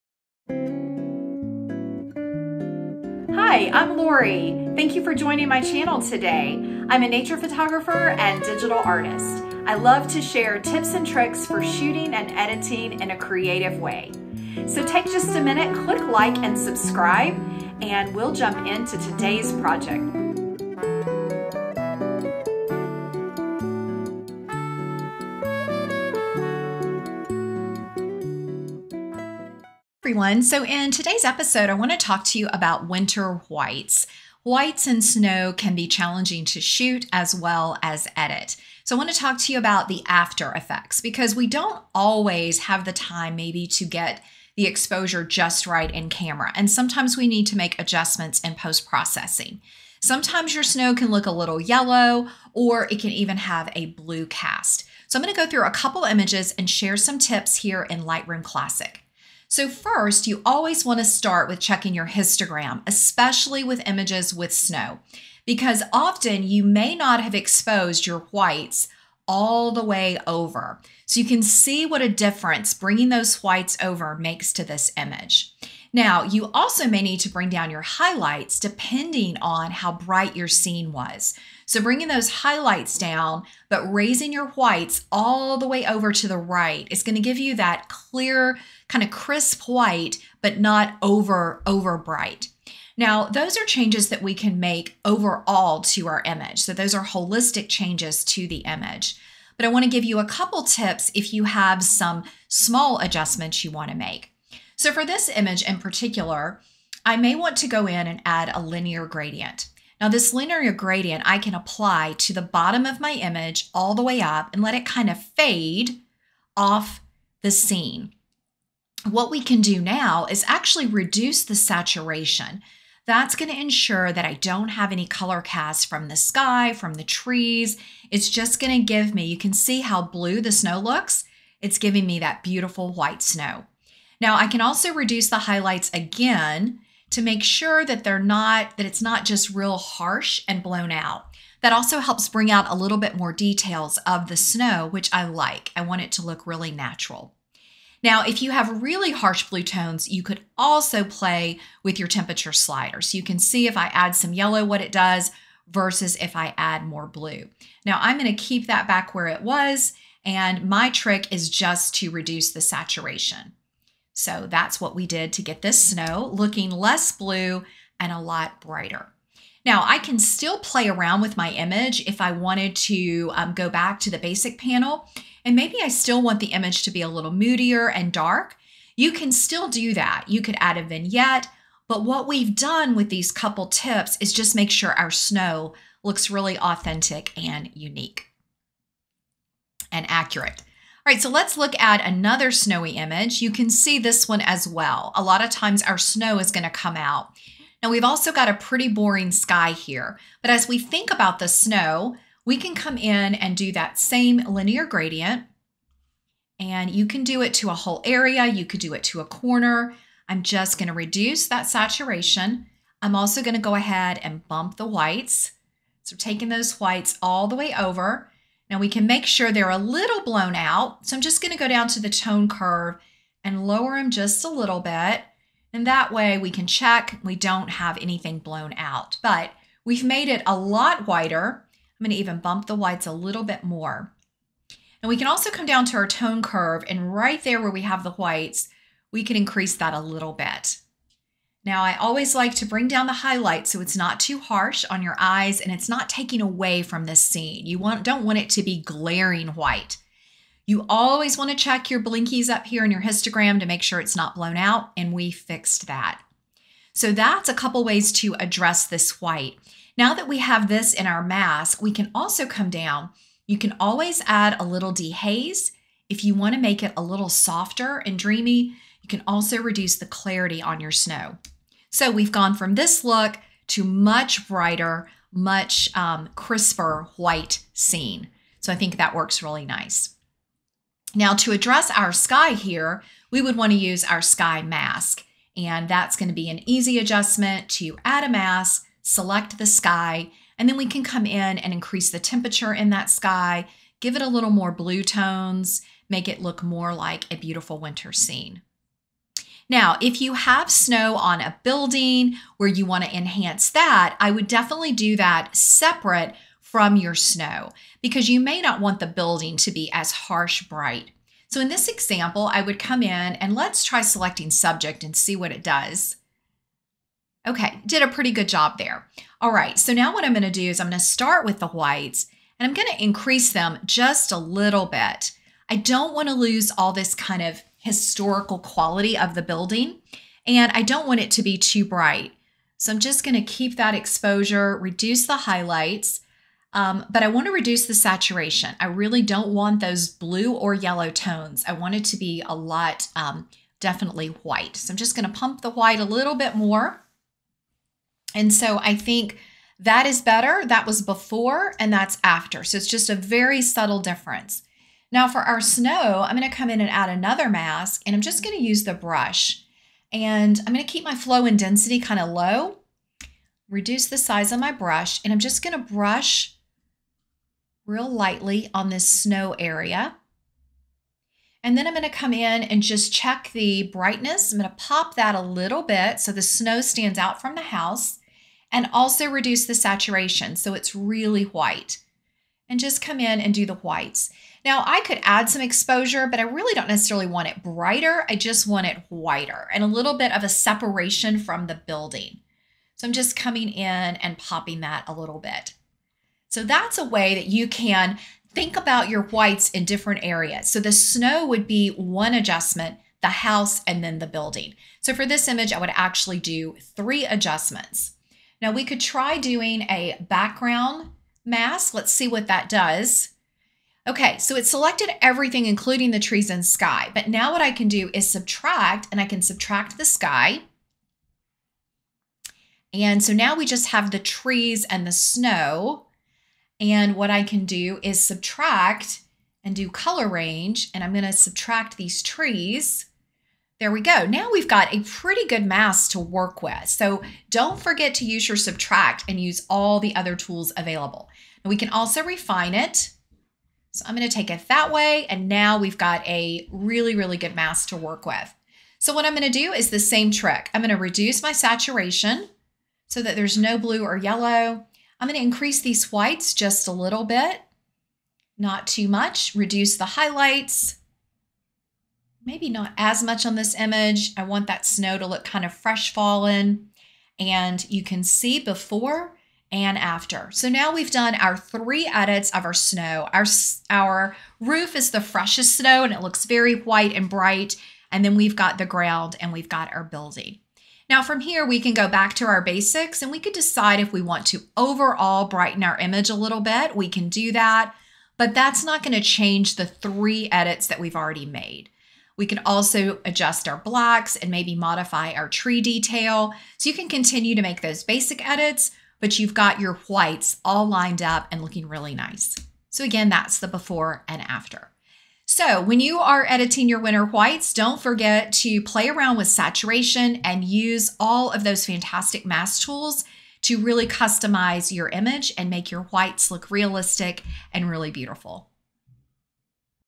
Master editing winter whites using tools in Lightroom | Editing tutorial
Winter whites can be challenging to capture and edit. In this episode, I will show some easy tips to reduce color saturation on your whites, brighten your whites in a natural way, and address different white colors on one image. We will edit using Lightroom Classic today.

▶️ Amazon Store for photo tools